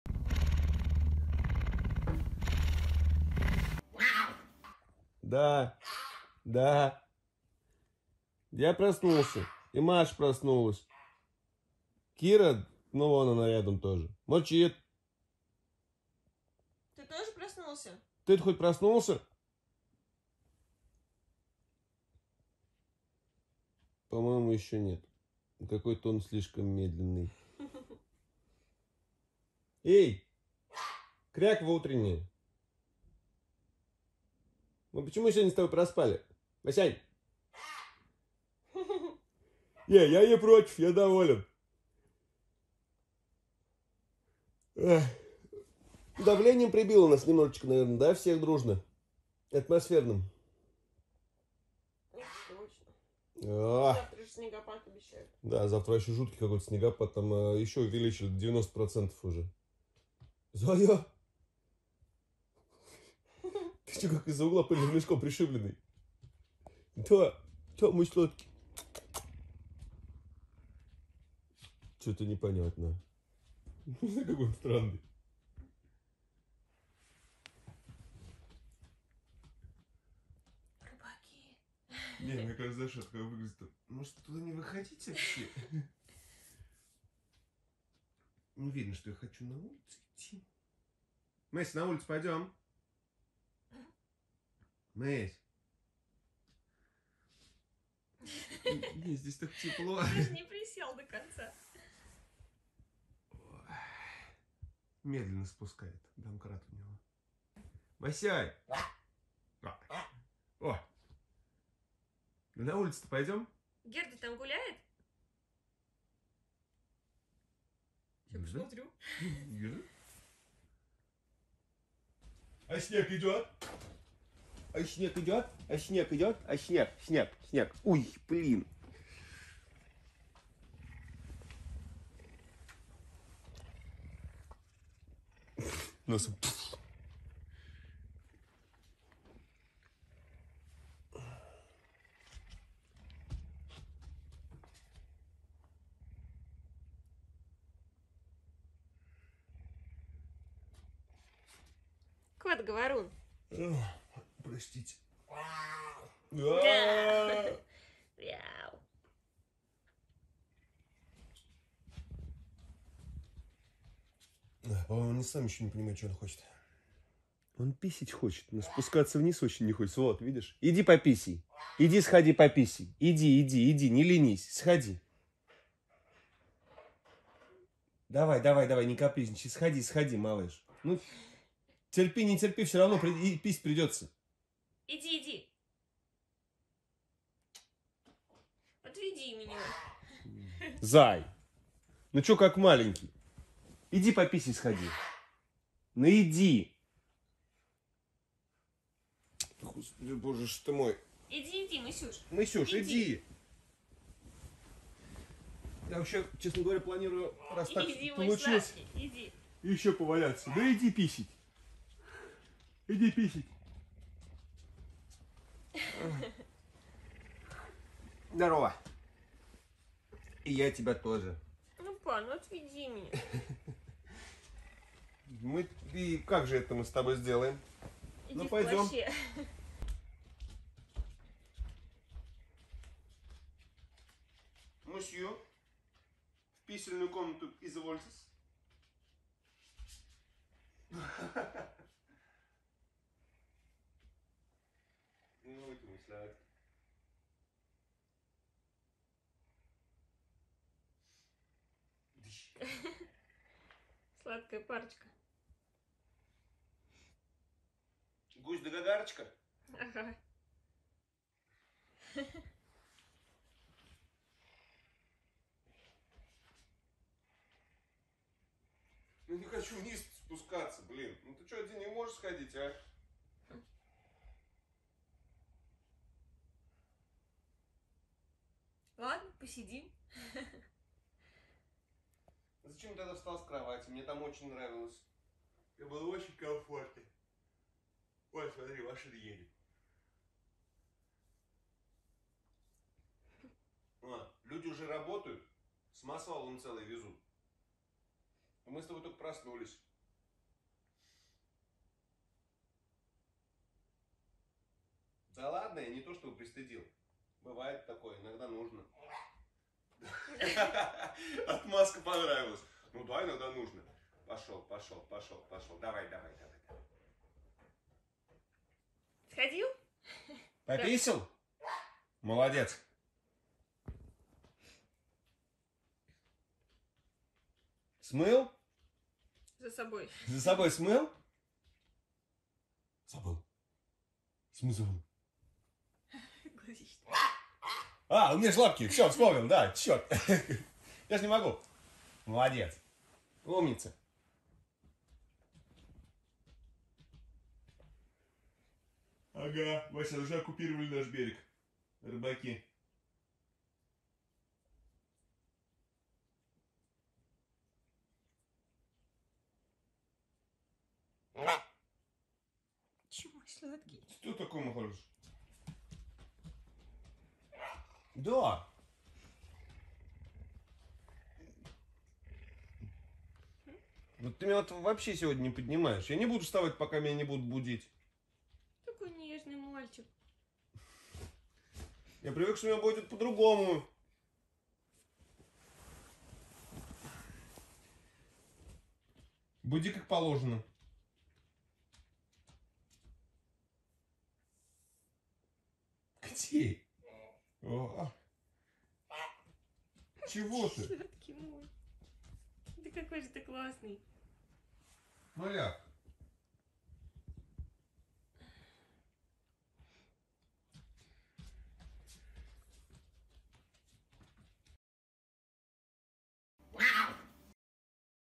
Месси проспал прогулку
Бывает и такое, проспали)) И он ведь в самом деле разговаривает с нами!